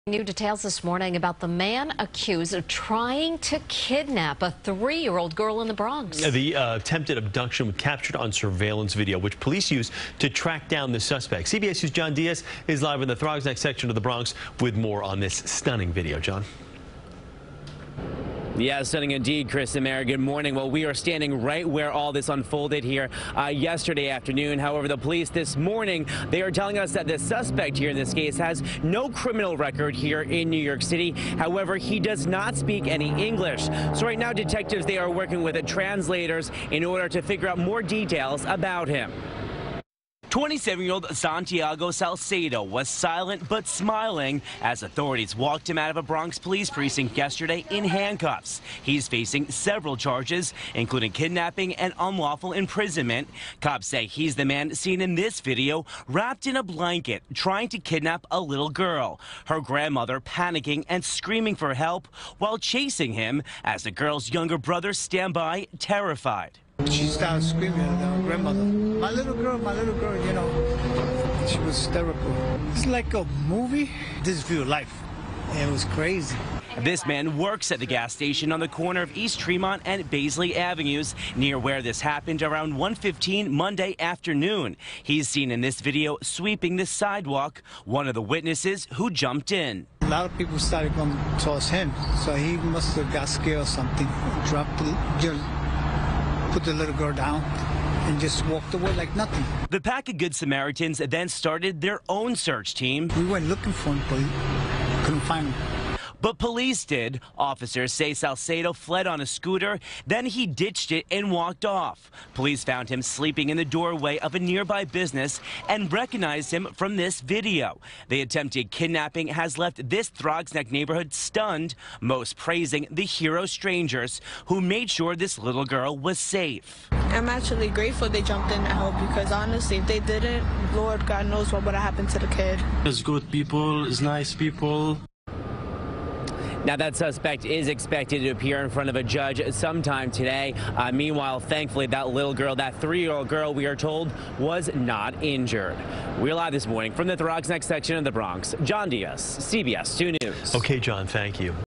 Bronx Attempted Kidnapping Suspect Santiago Salcedo All Smiles As He Leaves Precinct In Handcuffs
As CBS2's John Dias reports, 27-year-old Santiago Salcedo was silent but smiling as authorities walked him out of a Bronx police precinct Monday in handcuffs. He's facing several charges, including kidnapping and unlawful imprisonment.